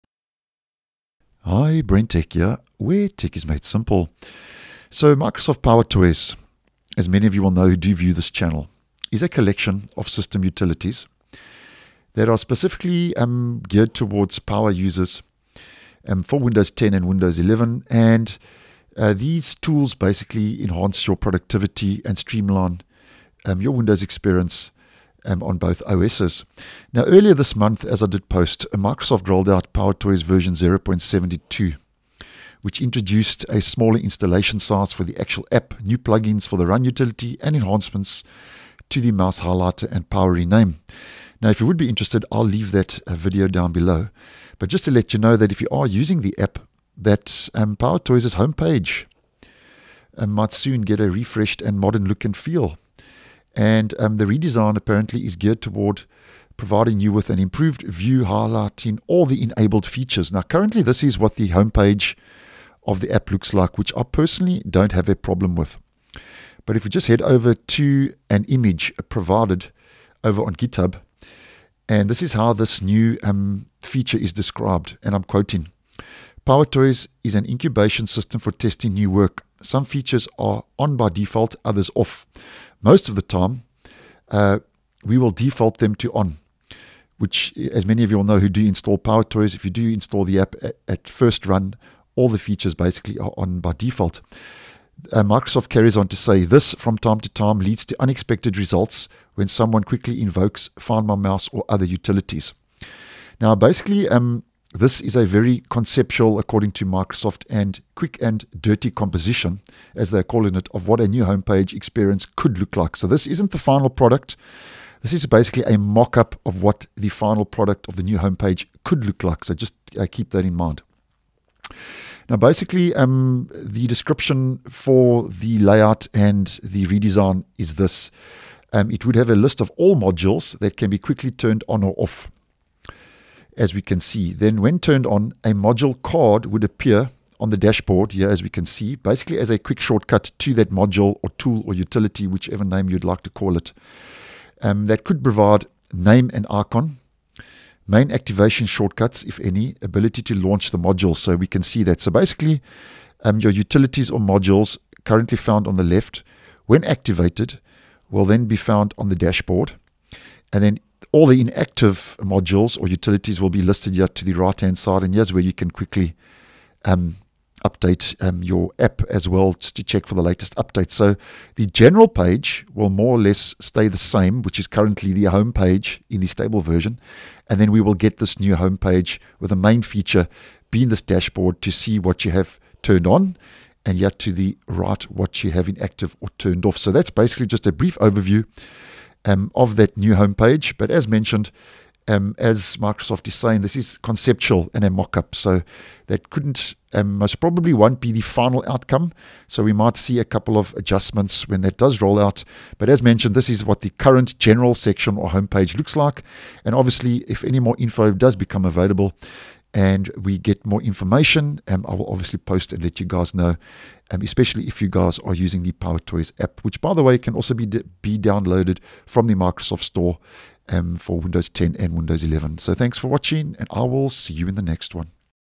Microsoft PowerToys could soon get a new Modern and Redesigned Homepage
PowerToy's homepage may soon get a refreshed and modern look and feel, geared toward providing you with an improved layout of all the enabled features.